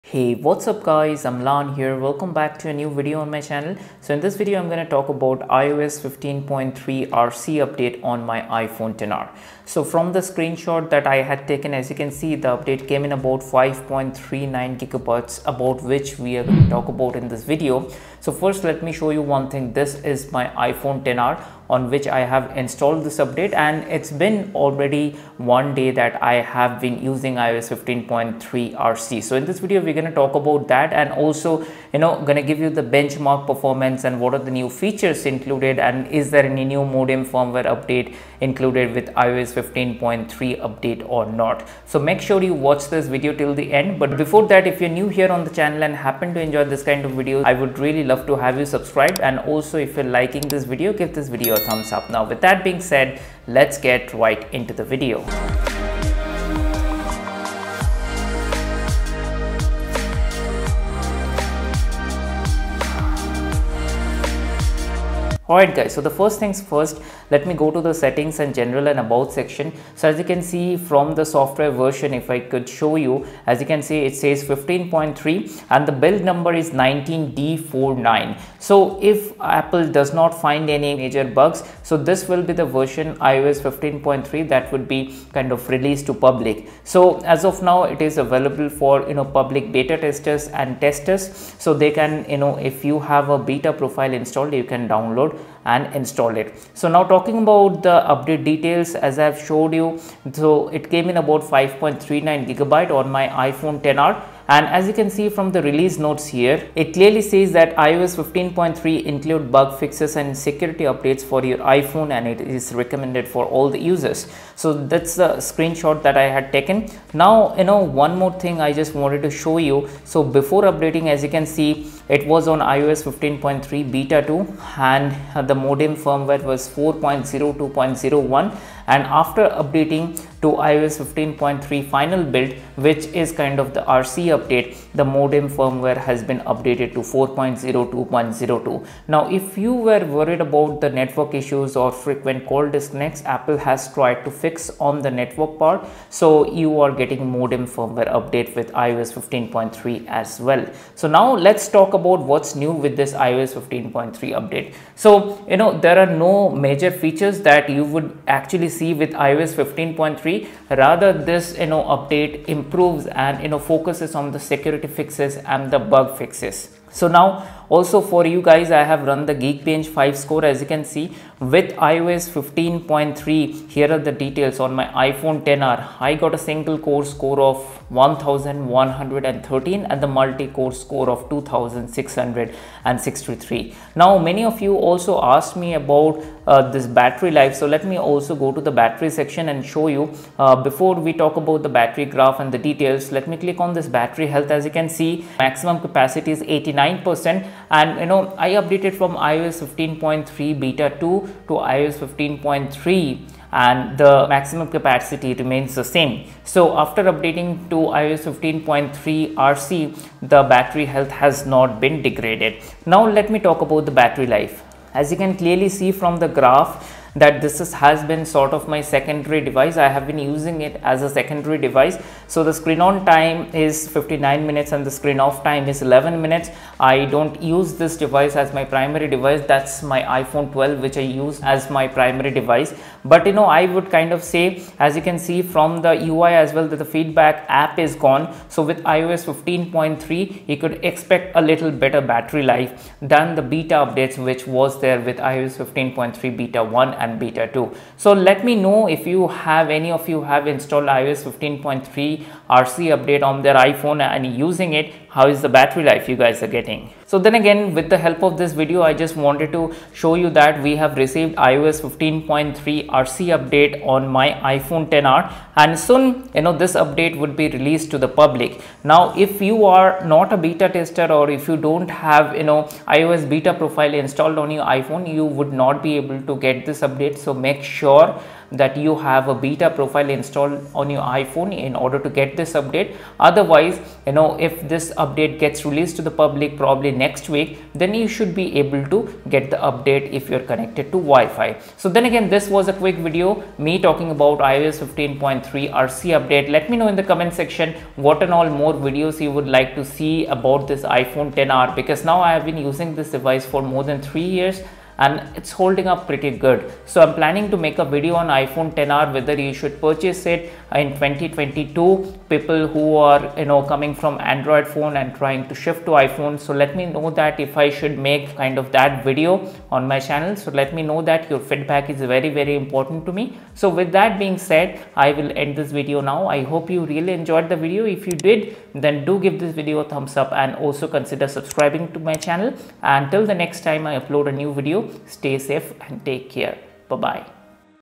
iOS 15.3 - RC Update on iPhone XR !
iOS 15.3 RC Update has been released for all iOS 15 Beta Testers. In this video, I have covered the details of iOS 15.3 RC update on my iPhone XR. Also Given the details, of Benchmarking scores and Performance after updating to iOS 15.3 RC. Watch the video till the end, if you want to know about latest iOS 15.3 update which will be released to Public soon.